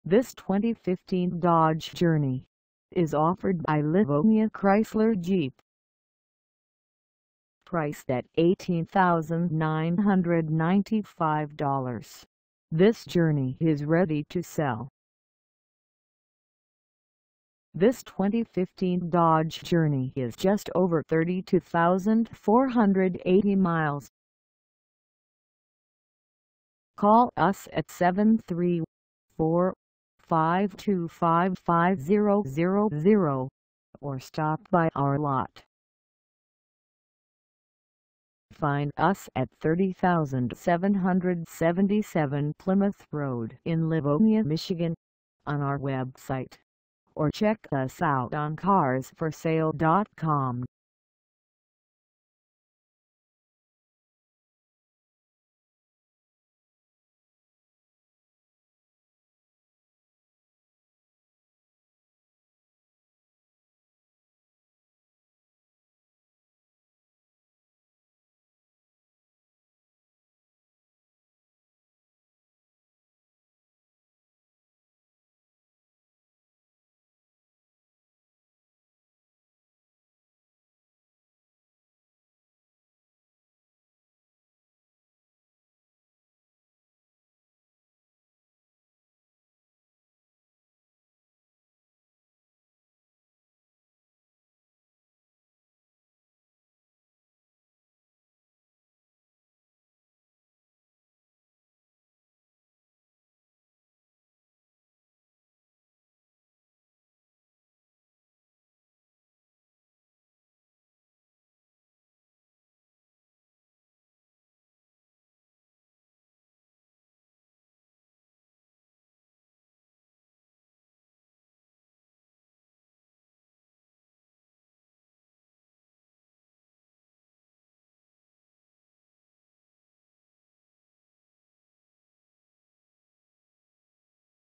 2015 Dodge Journey R/T AWD 4dr SUV for sale in Livonia, MI 4
This 2015 Dodge Journey R/T AWD 4dr SUV is for sale in Livonia, MI 48150 at Livonia Chrysler Jeep.

Engine: 3.6L V6
Transmission: Automatic 6-Speed
Color: Pitch Black Clearcoat
Mileage: 32480
Contact Livonia Chrysler Jeep 30777 Plymouth Rd Livonia, MI 48150 to test drive this 2015 Dodge Journey R/T AWD 4dr SUV today.